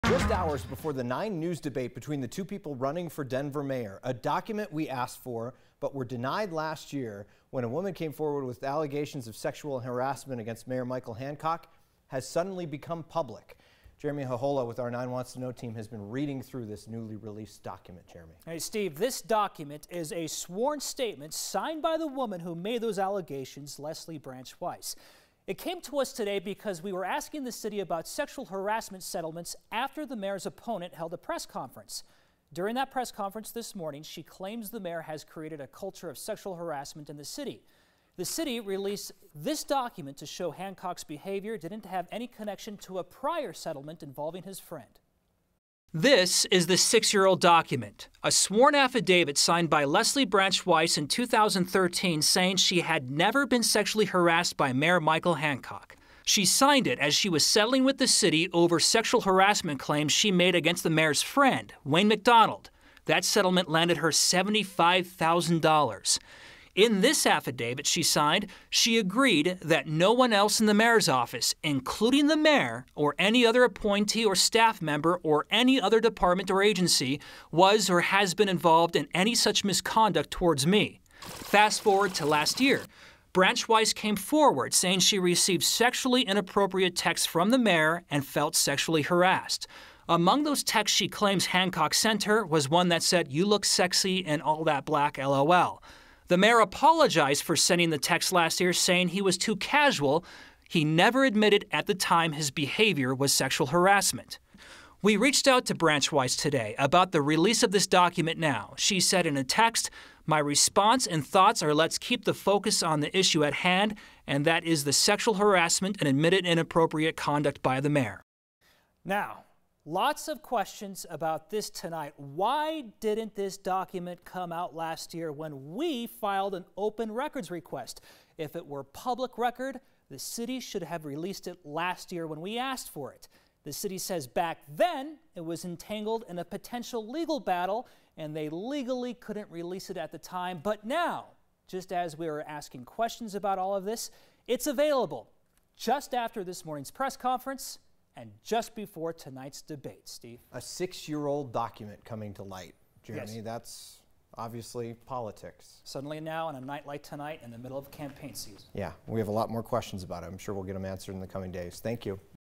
6-year-old document regarding sexual harassment claims against Denver mayor surfaces
Leslie Branch-Wise, the woman who accused Mayor Michael Hancock of sexual harassment last year, signed a sworn affidavit in 2013 that said she had never been sexually harassed by the mayor.

9NEWS (KUSA) is located in Denver, Colorado.